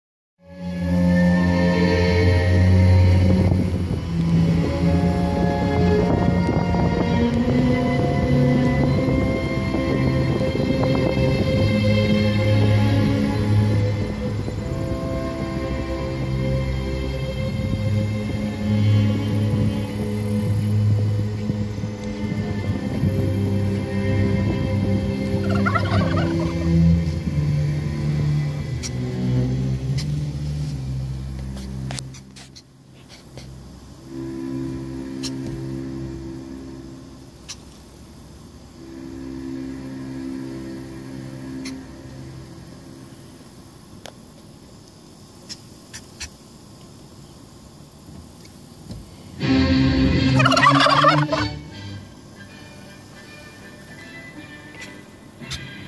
CONGESTED ROAD IN VILLA NOVA, ONTARIO
Traffic congestion on Villa Nova Rd. (heading south from Bealton) September 4, 2015. Soundtrack courtesy of CBC Radio 2.